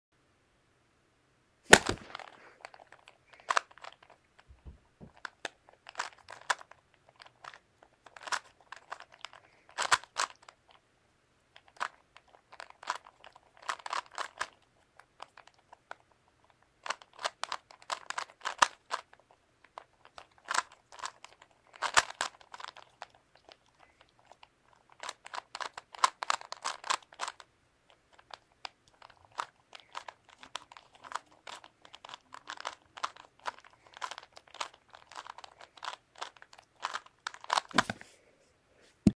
Solbing my first 2x2
I hope that you enjoy :) I love to solve cubes.